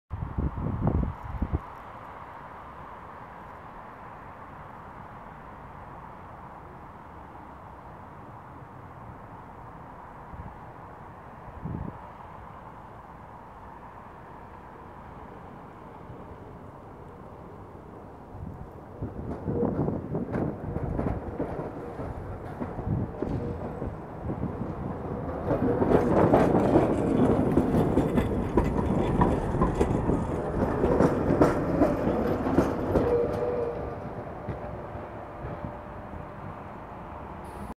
M31 linje 1 kör via Ekedal till Mstra sjukhuset vid Marklandsgatan
spårarbete påverkar spårvagnstrafiken vid marklandsgatan och då måste linje 1 köra via ekedal in till stan